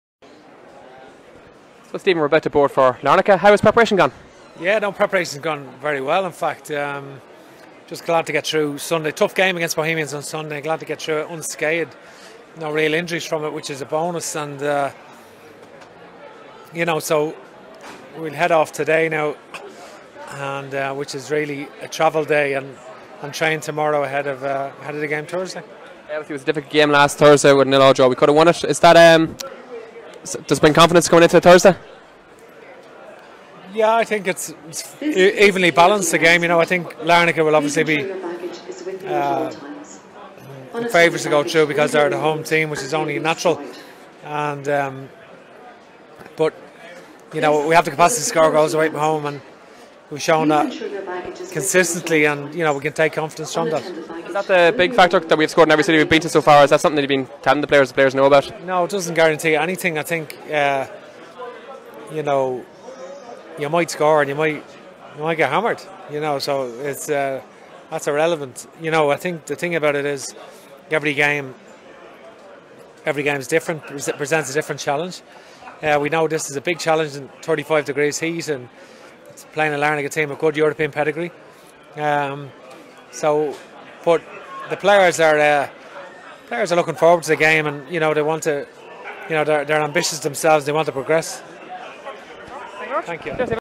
📹 DFCTV | Stephen Kenny: Pre-match Interview (Larnaca)
DundalkFC manager Stephen Kenny shares his thoughts with DFCTV before take-off at Dublin Airport.